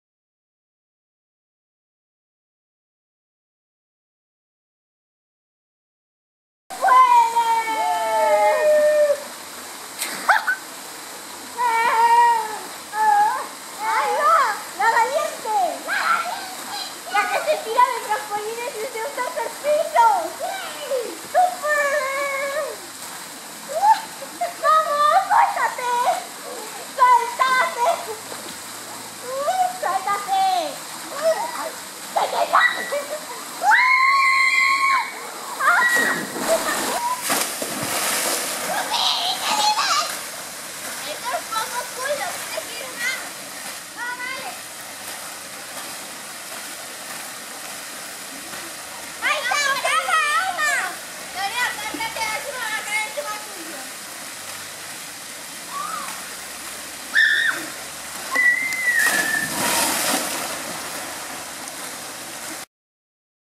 En la piscina de D Varee Diva Nana Resort
Lorea, Amaia y Maite en la piscina de D Varee Diva Nana Resort, cerca del Parque Nacional Kaeng Krachan (Tailandia) el 27 de julio de 2017.